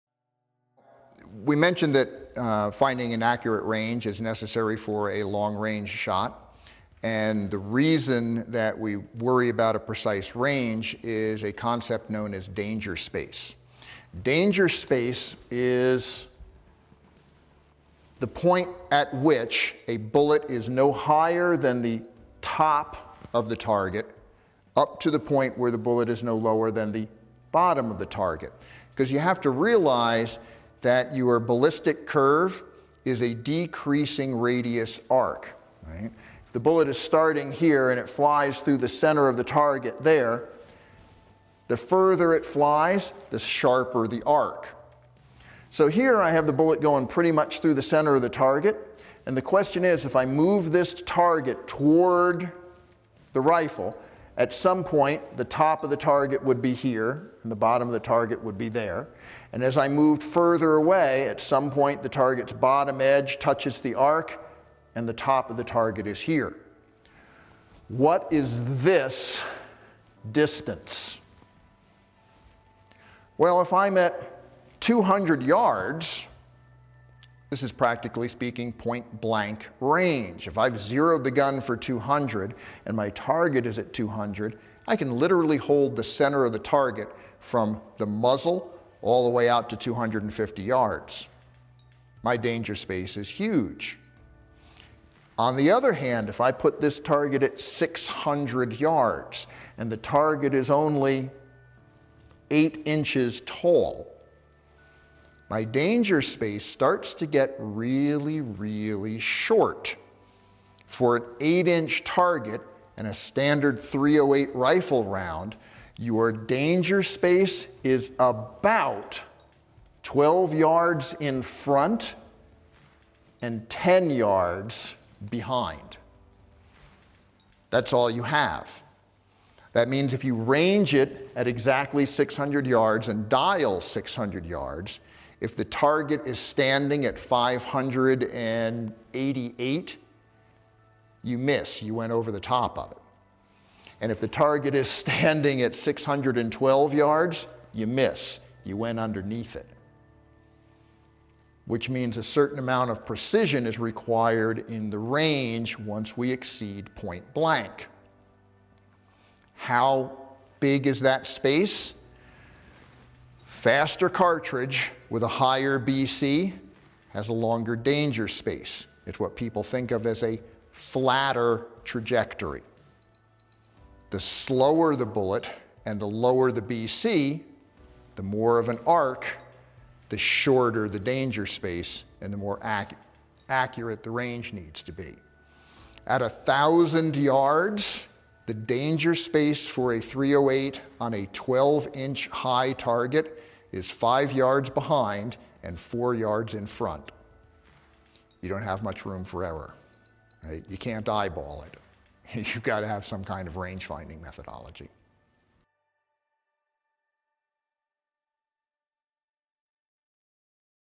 Defining Danger Space
Gunsite Academy Range Master, Cory Trapp explains the concept of Danger Space and why it's important to consider when making a long range shot. Check out Gunsite Academy to learn more about long range shooting and other shooting courses